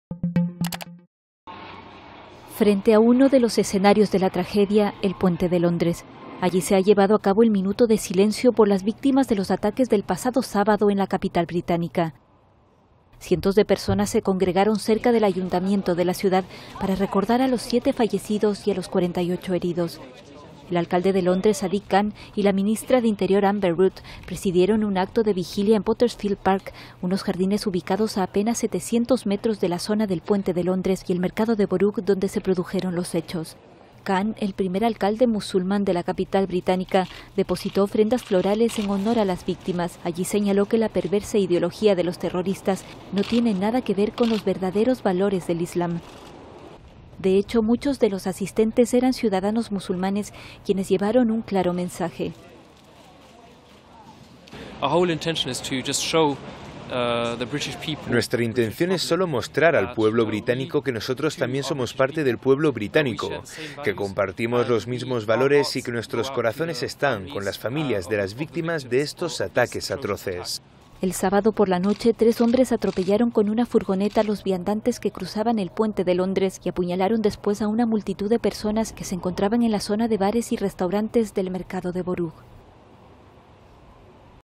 Londres recuerda, cerca del London Bridge, a las víctimas de los ataques
Londres (Reino Unido), 5 jun (EFE/EPA).- (Imagen Andre Pain) Cientos de personas se congregaron hoy cerca del Ayuntamiento de Londres para guardar un minuto de silencio en honor a las siete víctimas mortales y los 48 heridos que dejó el ataque terrorista del sábado por la noche en la capital británica.